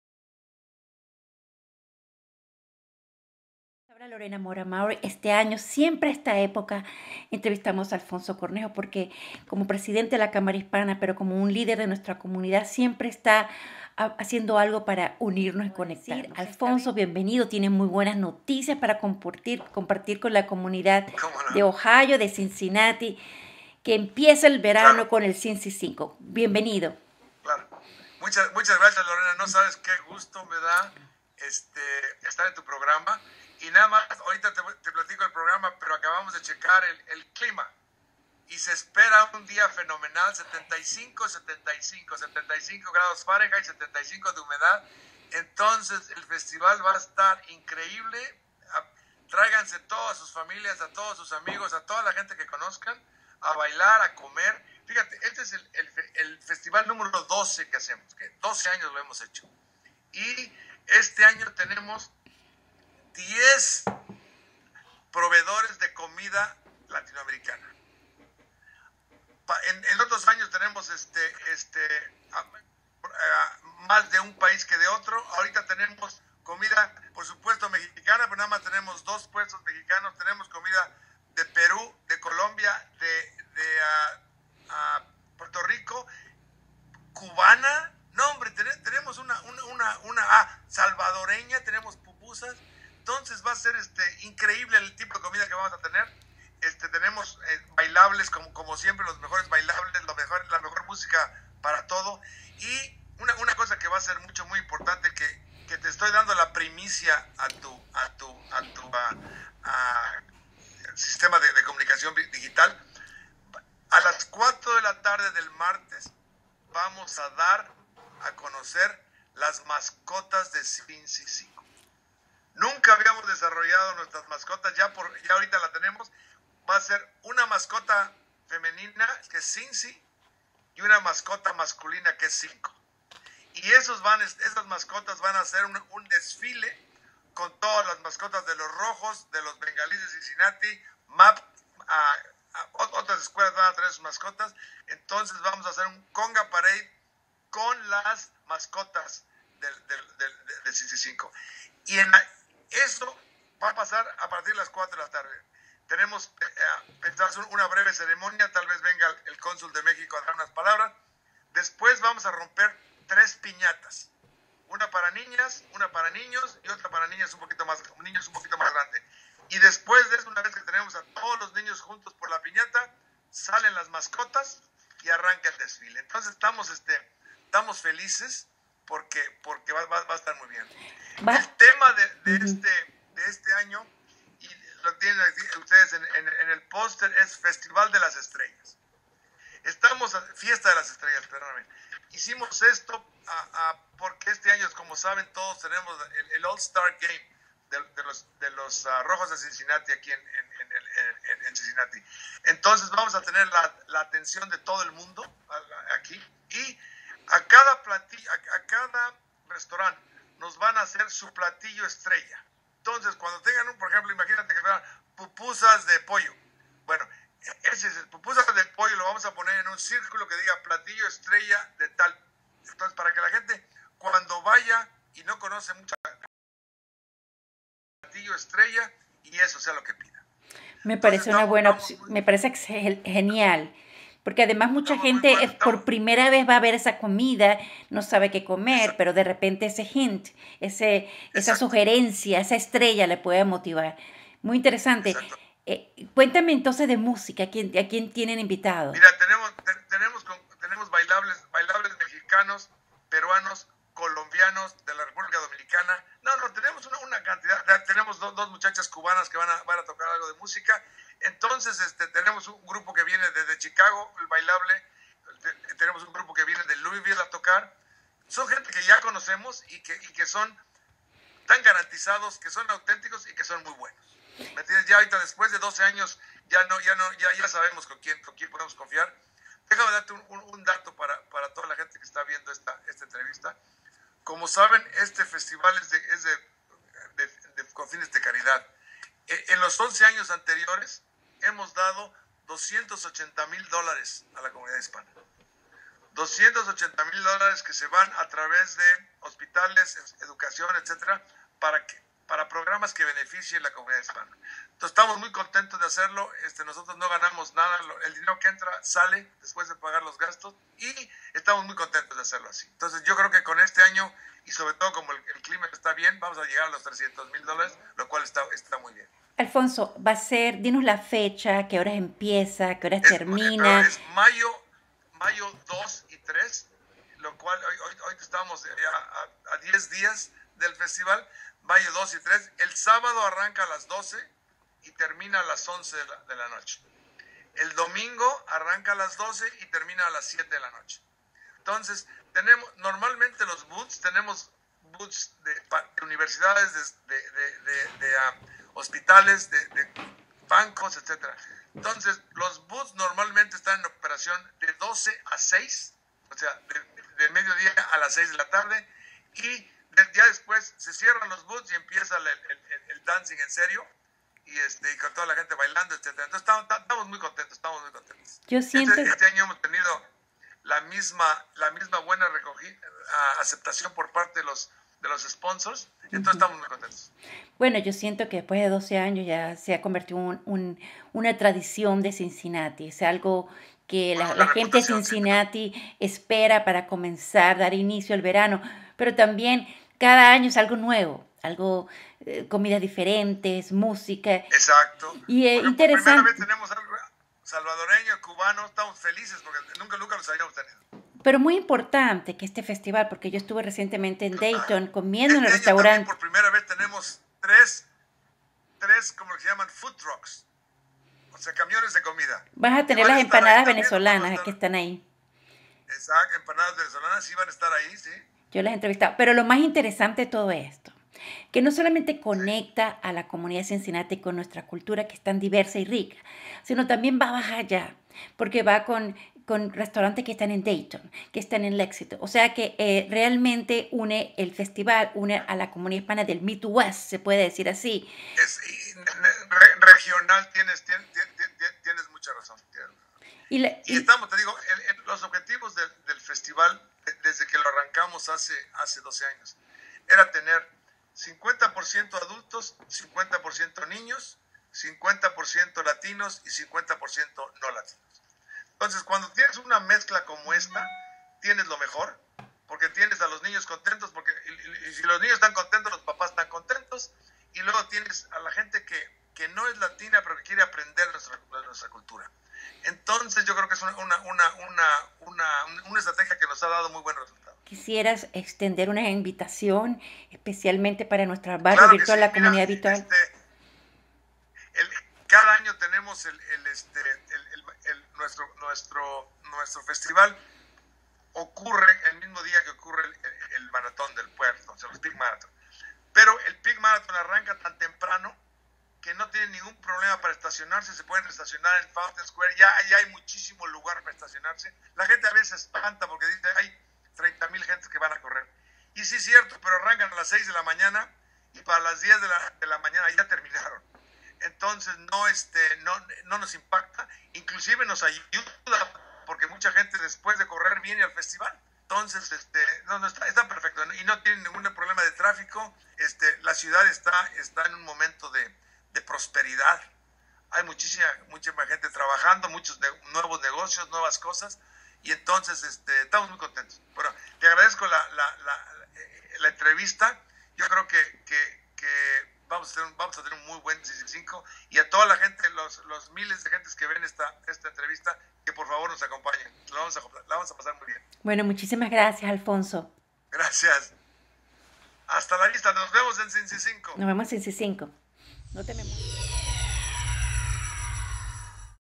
Festival Cincy-Cinco 2015, Cincinnati, Ohio
Alfonso Cornejo, presidente Hispanic Chamber Cincinnati USA los invita al Festival Cincy-Cinco 2015 el 2 y 3 de Mayo 2015 en el Fountain Square, Cincinnati.